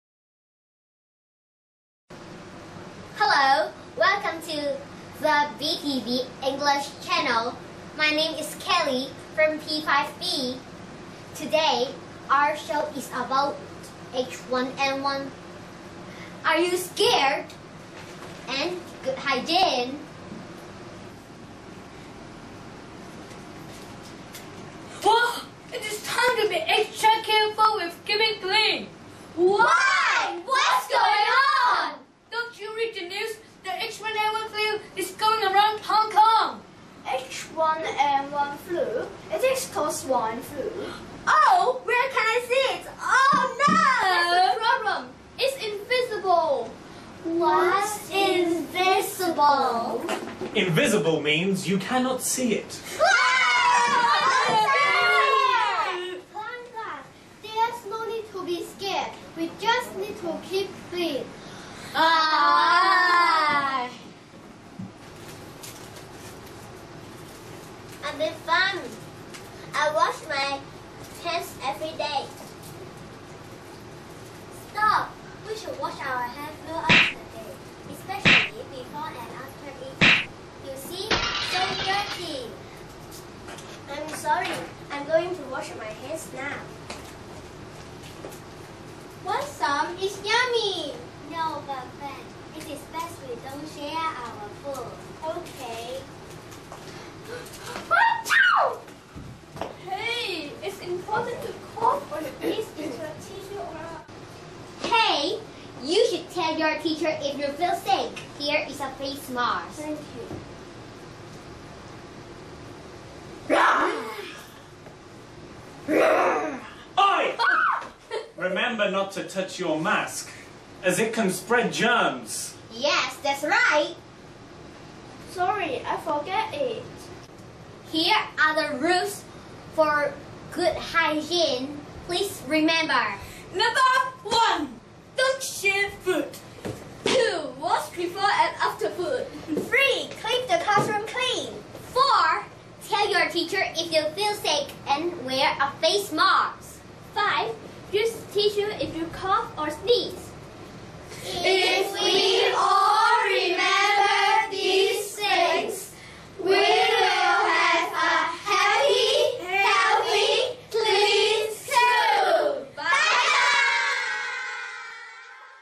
Tom's TEFL - Good Hygiene Role Play
Reminders for students amidst the H1N1 outbreak. All shot in our blue screen TV studio using real backdrops of our school in Hong Kong.